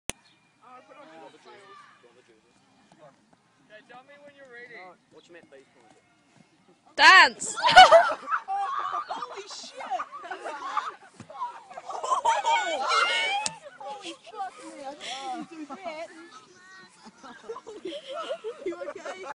Tire FAIL(original)
At school my mate made me fail and fall of this tire we were riding on!